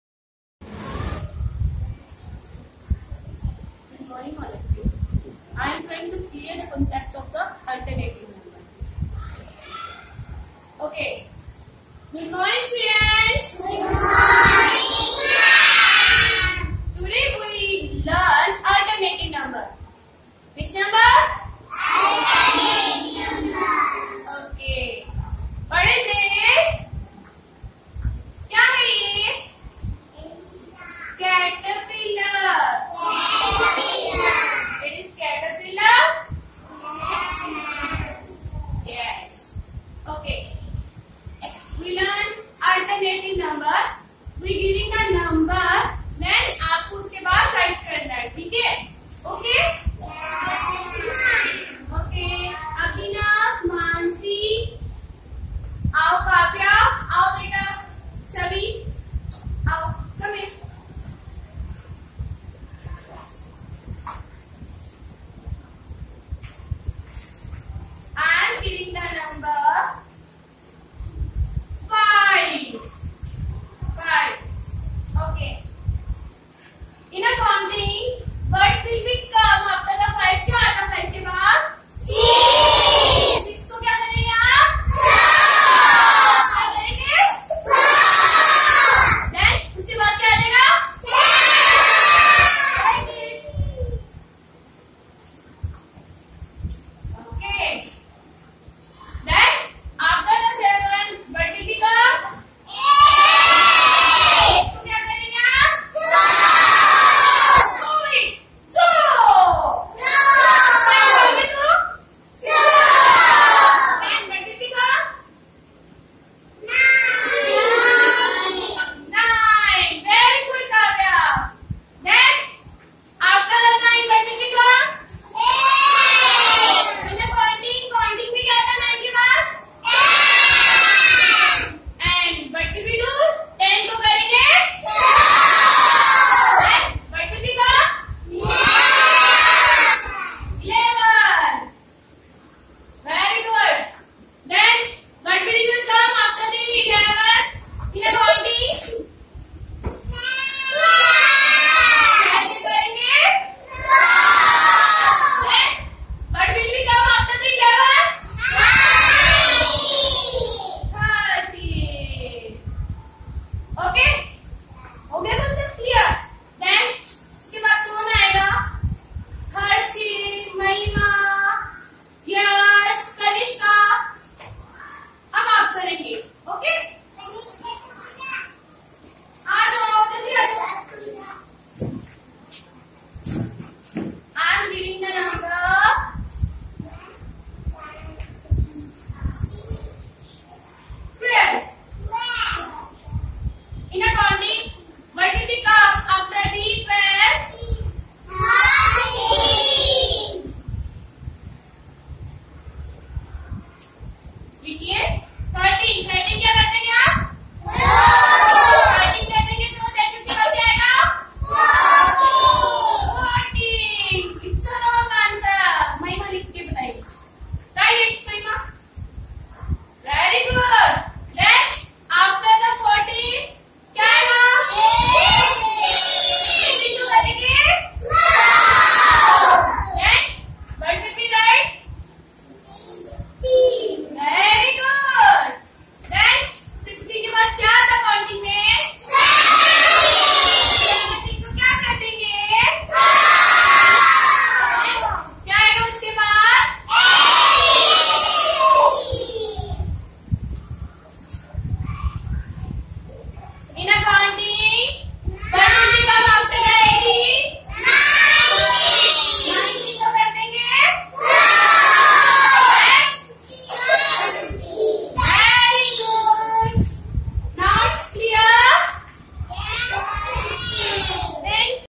SPS: Alternate Numbers activity by Grade 1
Learning done in a fun manner is enjoyable to both teacher and students. Teacher of grade 1 introduced the caterpillar activity for better understanding of the concept.